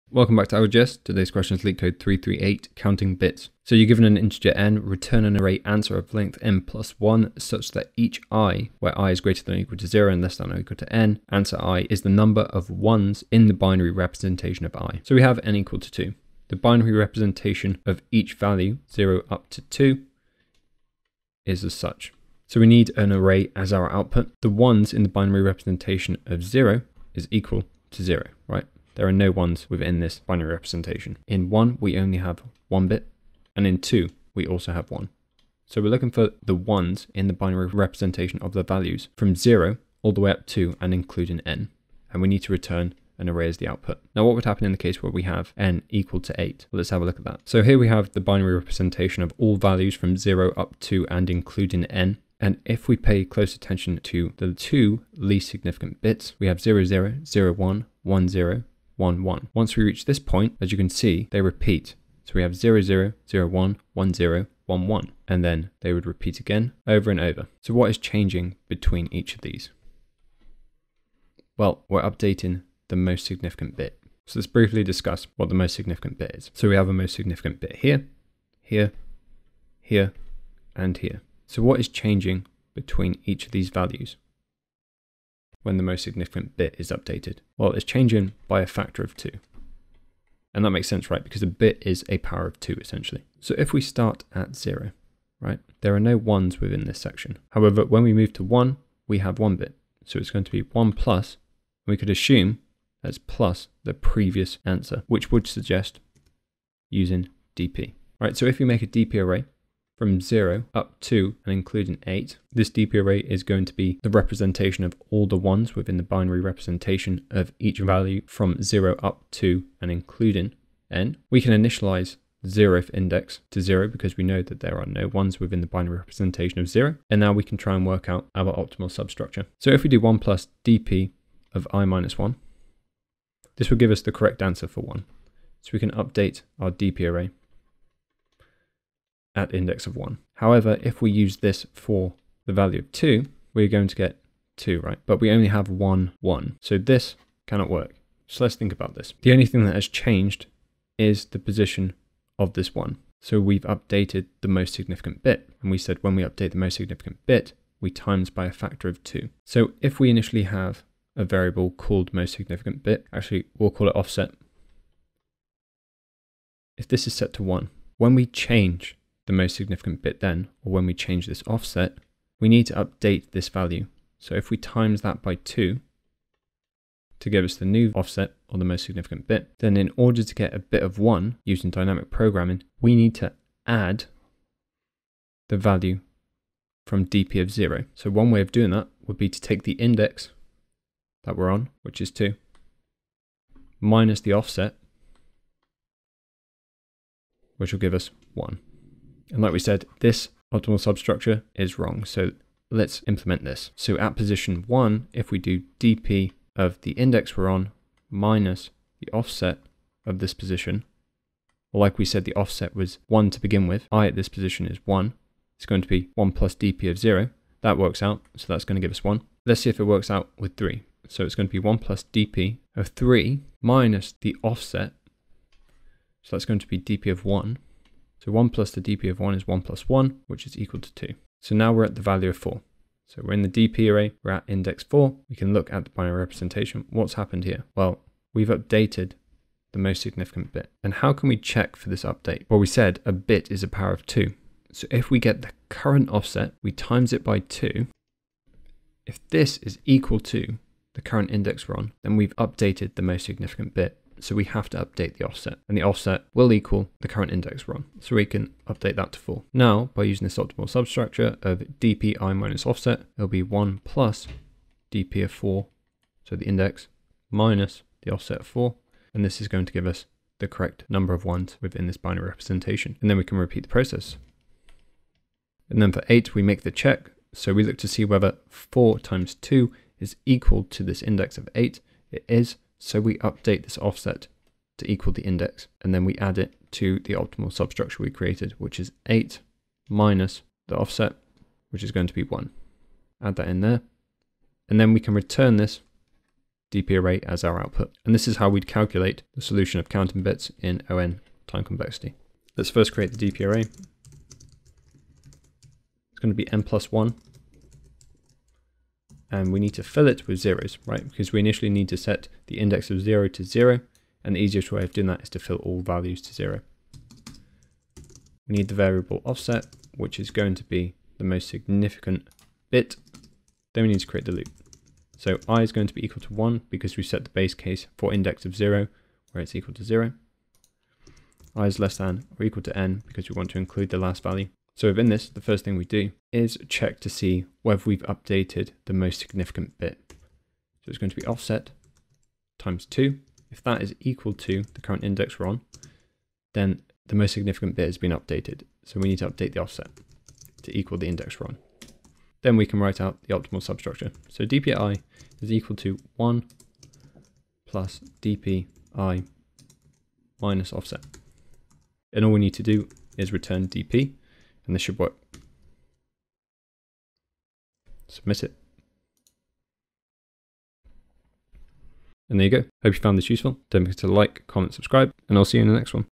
Microsoft Coding Interview Question - Counting Bits - LeetCode 338
Step by step walk through of the solution to the popular Microsoft coding interview question, Counting Bits.

LeetCode NUMBER

JavaScript

0:00 Intro
0:15 Explanation
5:03 Code